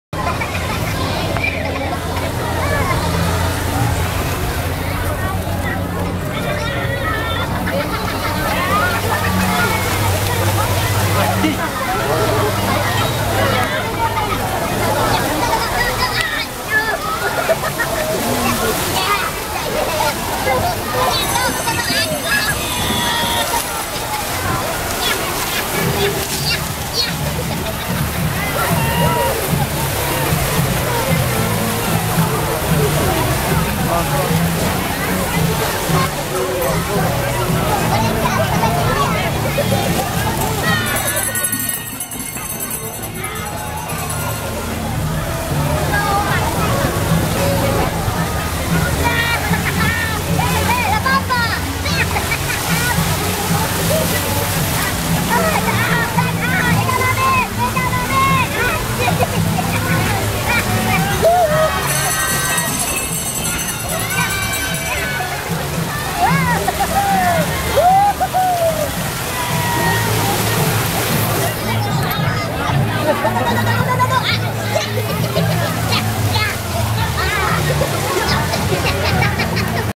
ユニバーサル・スタジオ・ジャパン ミニオン・パークの噴水
こちらはミニオン・パークがオープンした時に設置されました。日没前に撮影しました。